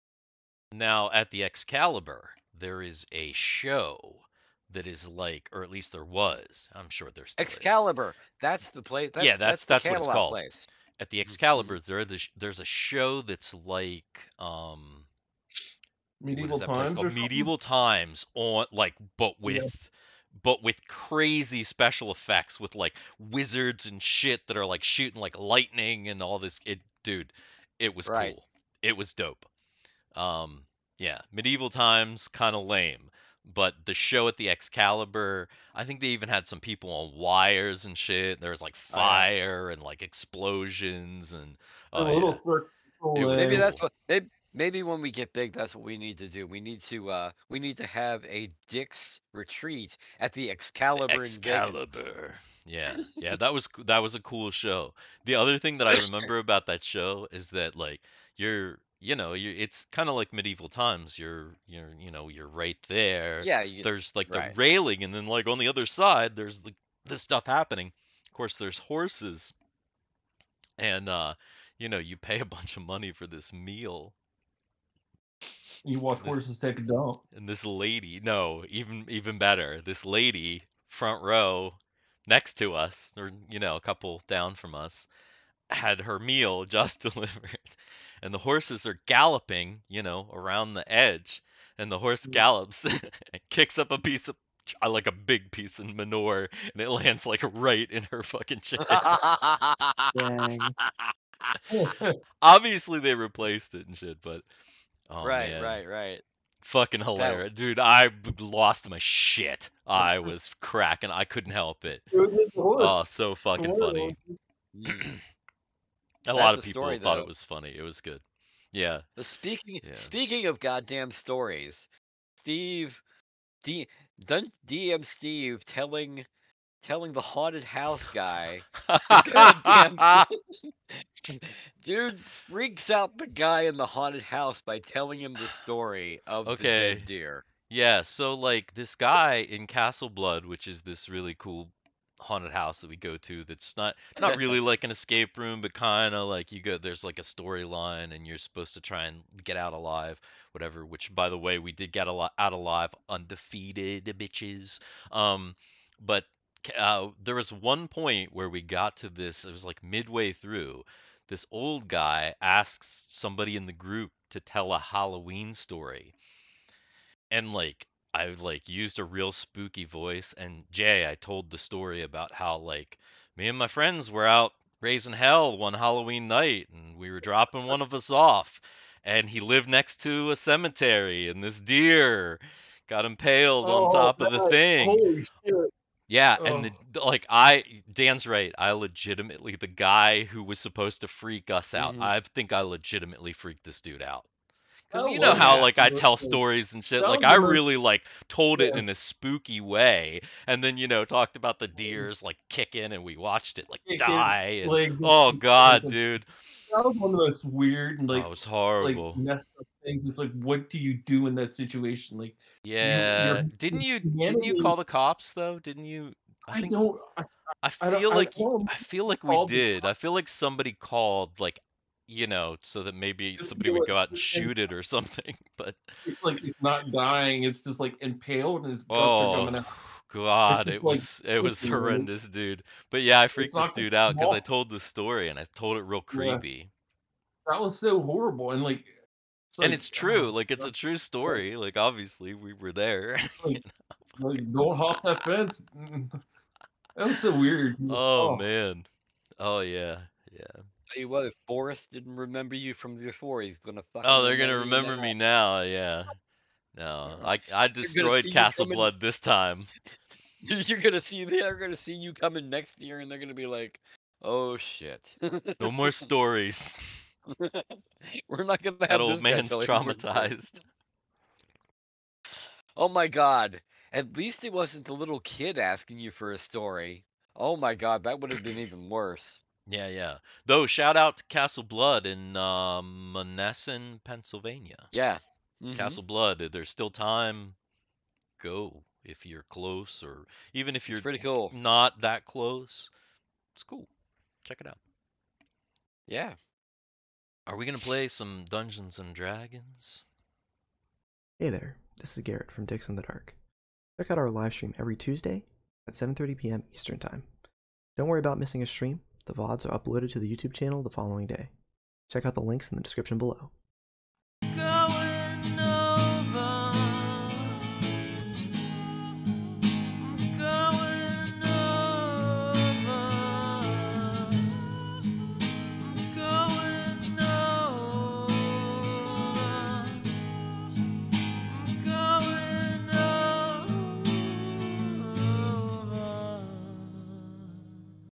"Excalibur & Castle Blood" | Funny Stories | An Outtake from our D&D Campaign Up, Up, and Away!
DM Steve tells two funny stories: First a lady's dinner is ruined at the Excalibur in Las Vegas. Then a haunted house actor is creeped out by a real life Halloween tale.